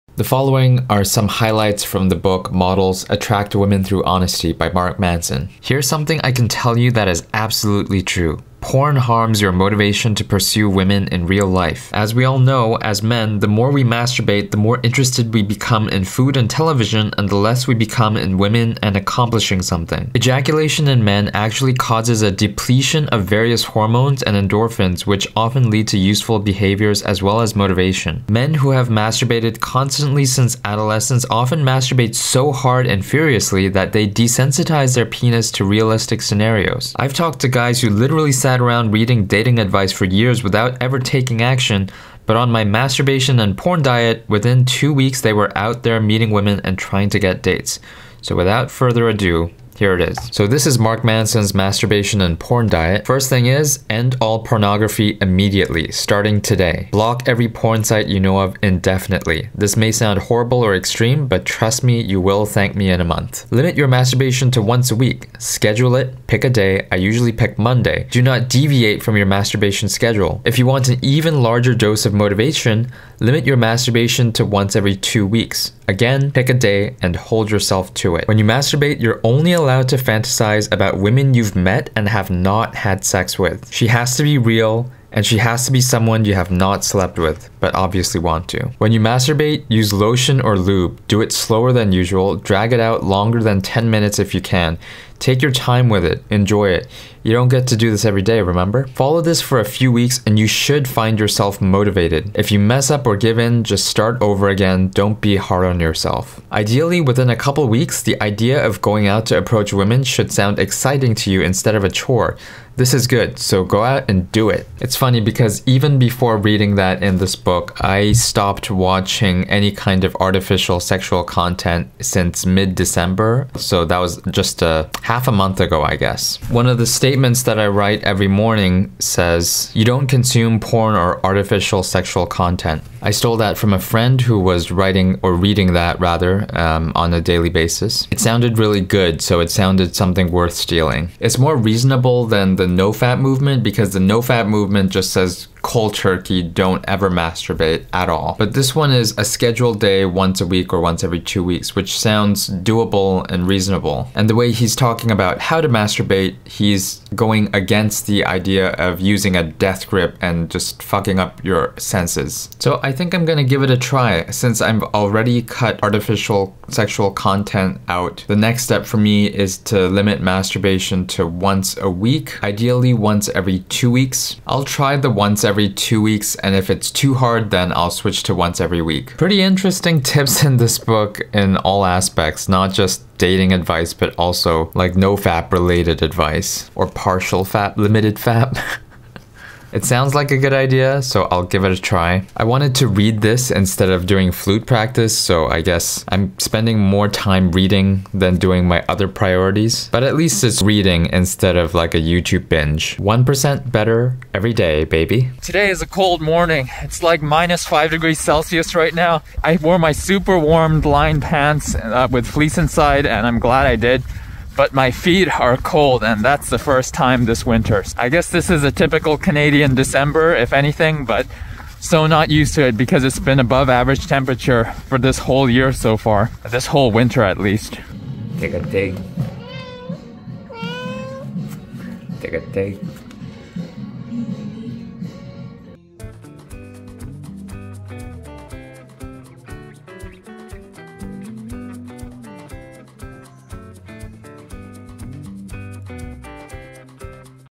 No More Artificial Sexual Content - 1% Better Day 67
I think this limited fap will be challenging, much more challenging than no porn.